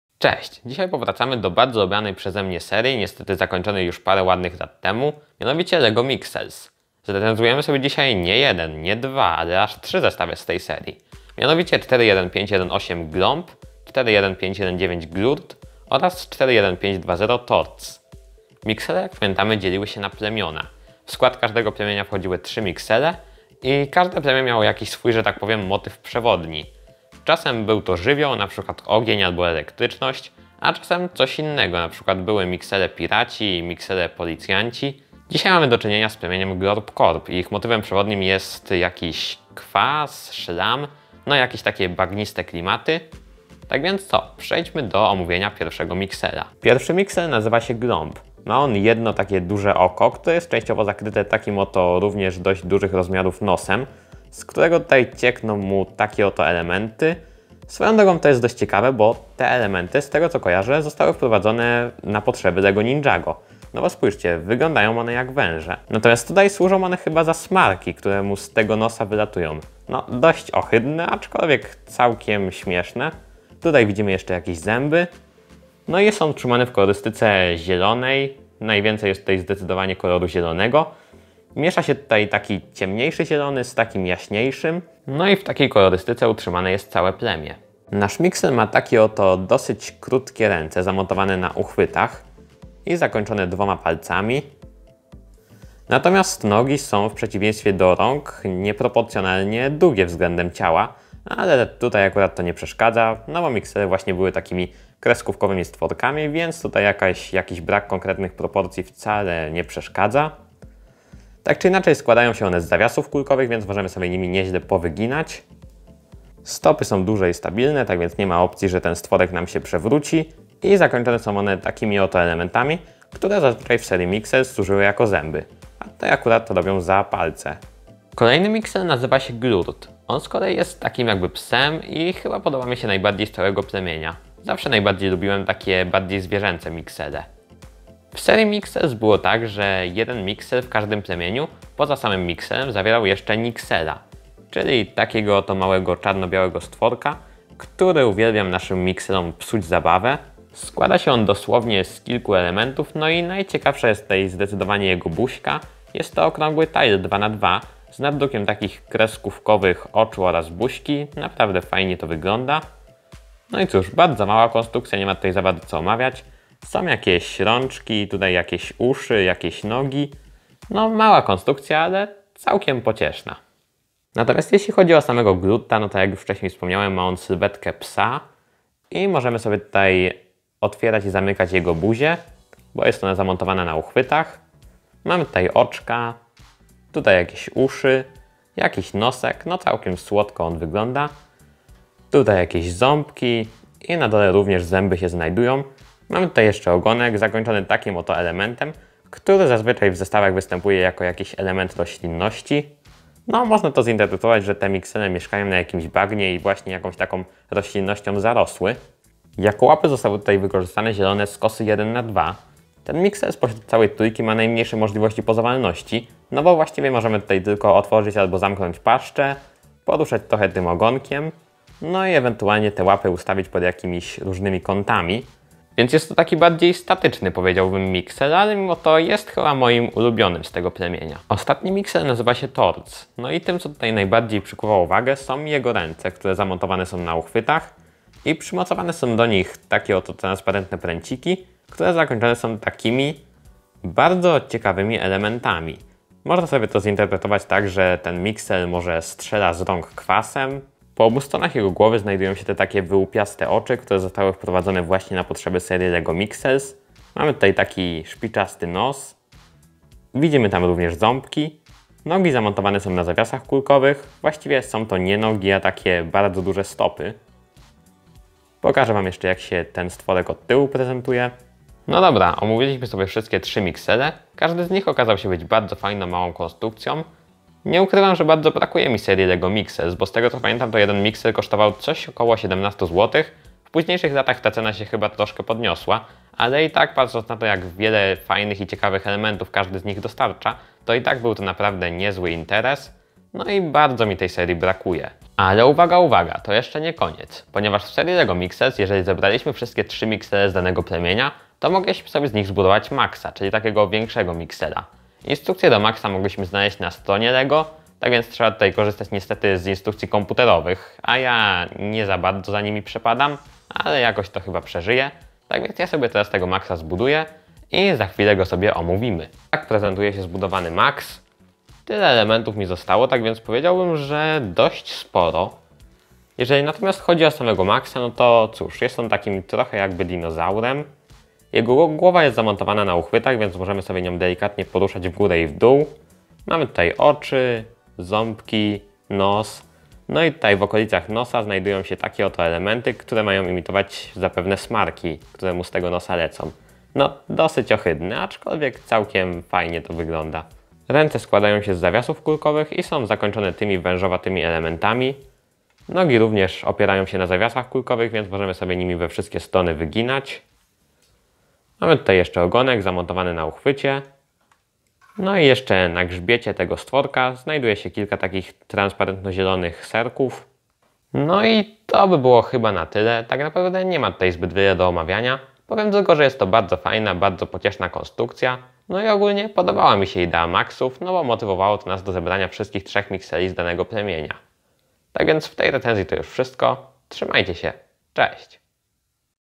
LEGO Mixels 41518 Glomp / 41519 Glurt / 41520 Torts / Recenzja
Glomp
numer zestawu:                       41518
liczba elementów:                   64
cena katalogowa:                    17,00 zł

Glurt
numer zestawu:                      41519
liczba elementów:                  62

Torts
numer zestawu:                      41520
liczba elementów:                  48